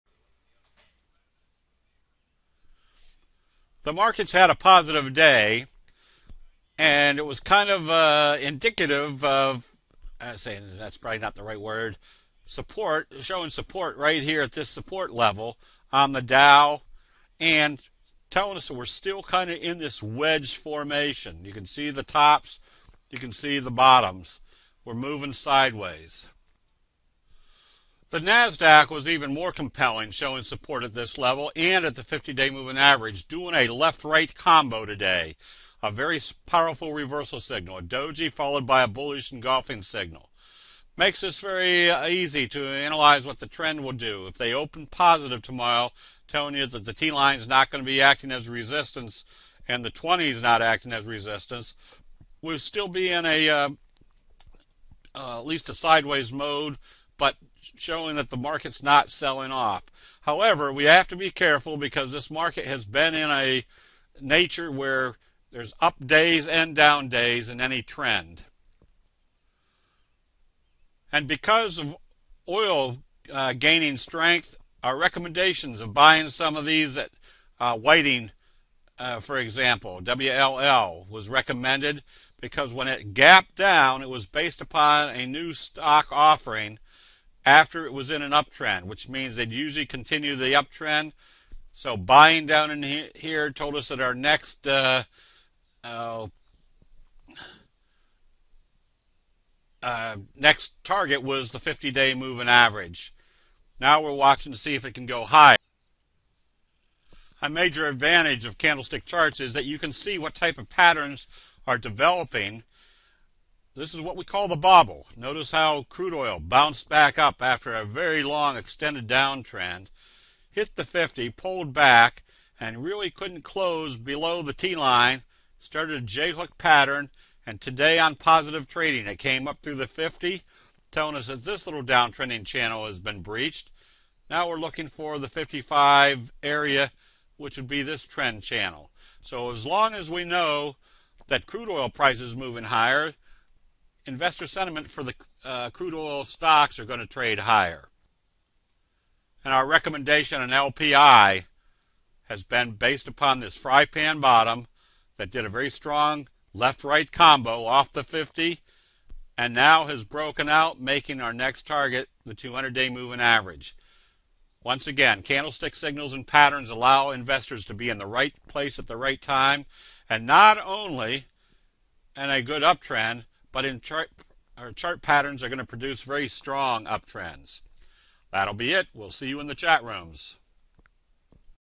candlestick forum market direction April 6, 2015a
The graphics of candlestick charts allows an investor to accurately analyze a market trend. This also allows investors to use simple scanning techniques to find the best sectors.